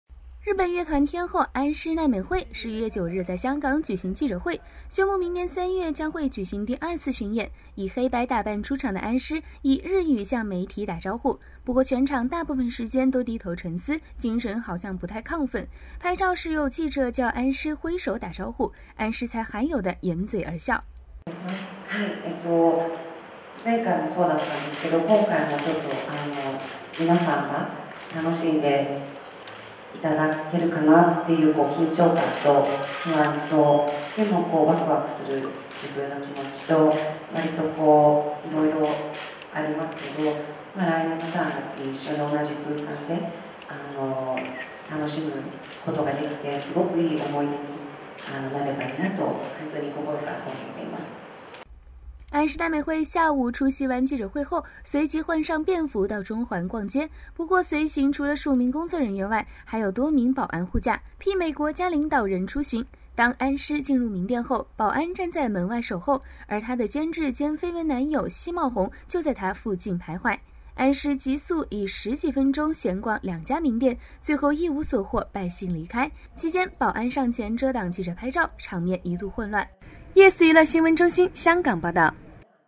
【4年前】安室奈美惠訪港血拼敗興而回 排場媲美領導人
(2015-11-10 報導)

日本樂壇天后安室奈美惠
11月9日在香港舉行記者會
宣佈明年三月將會舉行第二次巡演
以黑白打扮出場的安室
以日語向媒體打招呼
不過全場大部分時間都低頭沉思
精神好像不太亢奮
拍照時有記者叫安室揮手打招呼
安室才罕有的掩嘴而笑